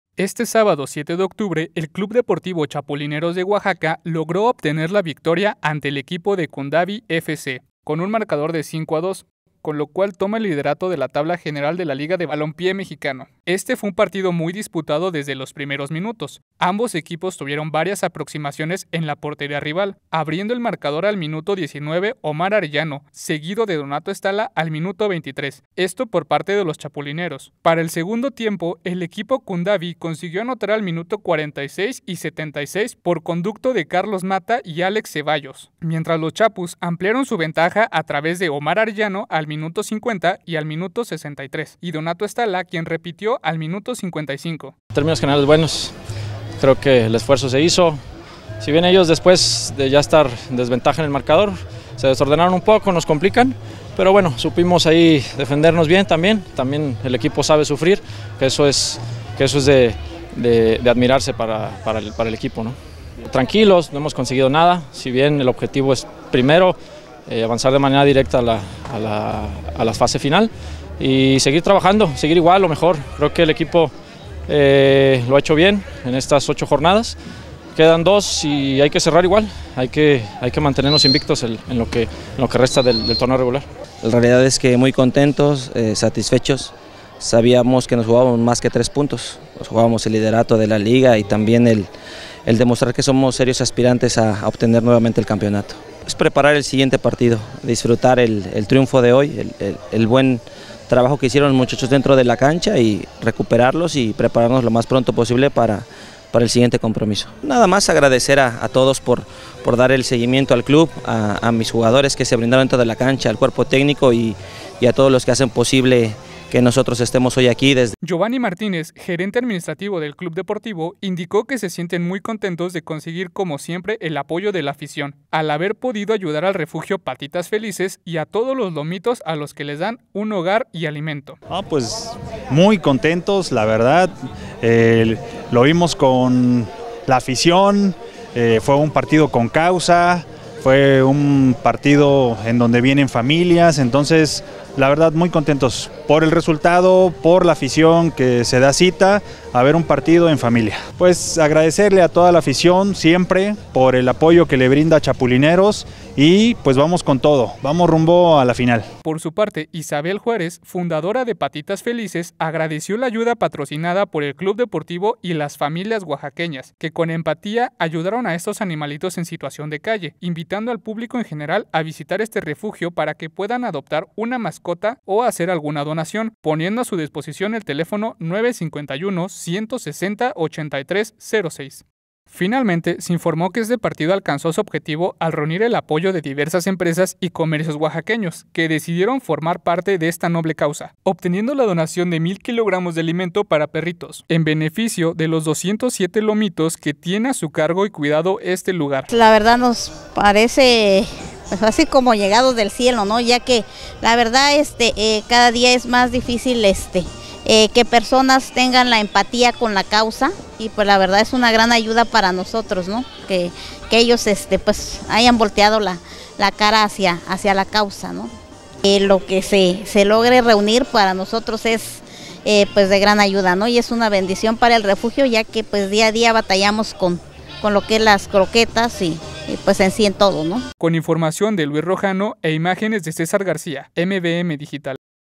CHAPULINEROS OBTIENEN VICTORIA 5 -2 CONTRA KUNDA´VI FC
🟦 Oaxaca | Los Chapulineros de Oaxaca lograron obtener la victoria ante el equipo Kunda' vi F. C. con un marcador 5 - 2, en un partido ConCausa en el que se alcanzó el objetivo de apoyar al refugio Patitas Felices.